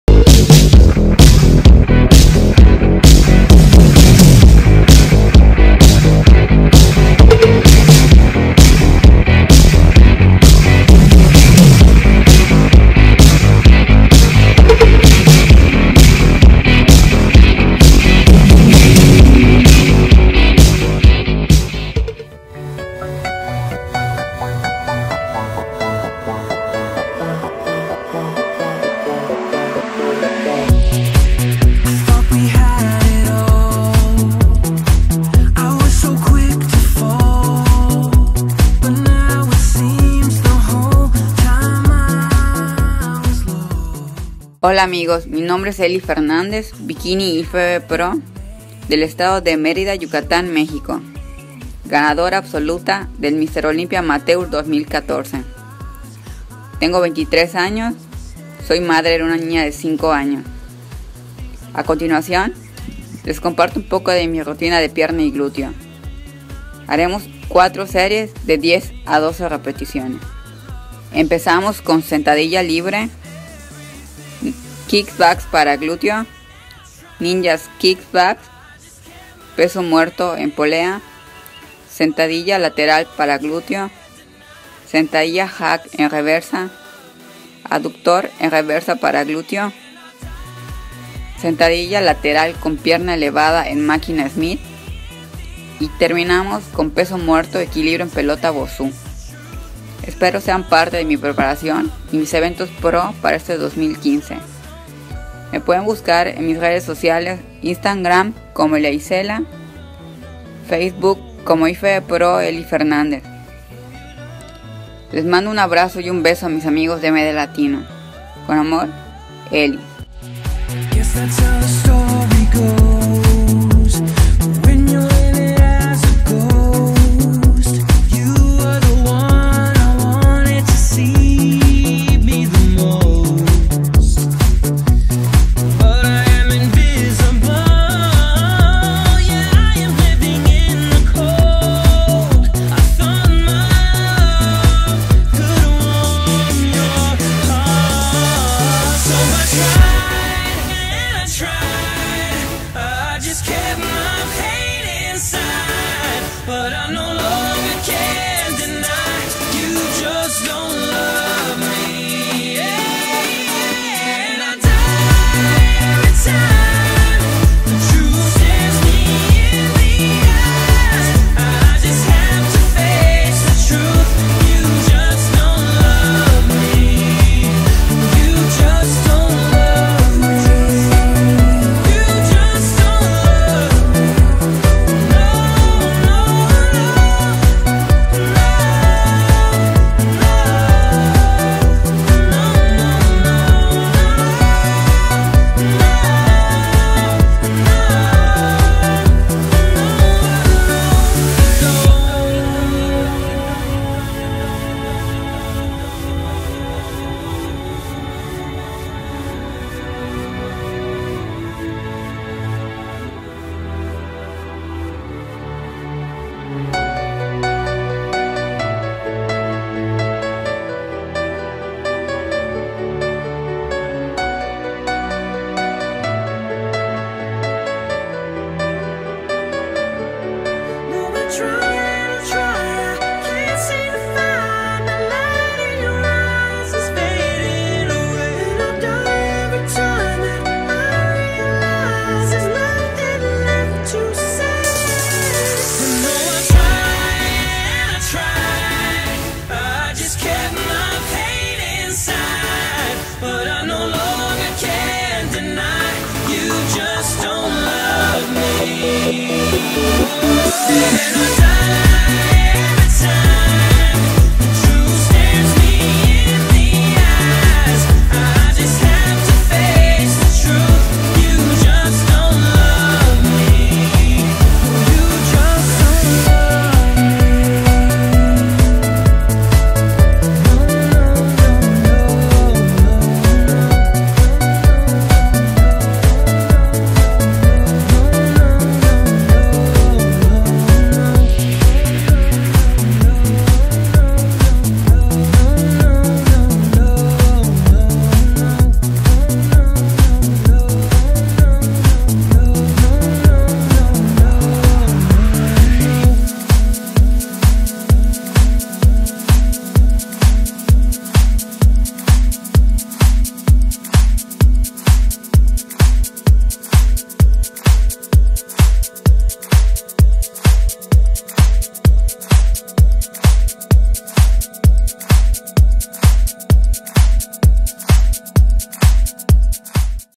Elia Fernandez IFBB Pro Bikini Fitness Mexico
IFBB Bikini Pro Elia Fernandez de Merida Mexico dispone su mejor rutina de pierna y gluteo para los lectores MD Latina en su temporada libre y comenzando su preperacion para competencias pro 2015. Elia habia ganado su carnet en el mr. olympia amatuer 2014. carrito.mdlatino.com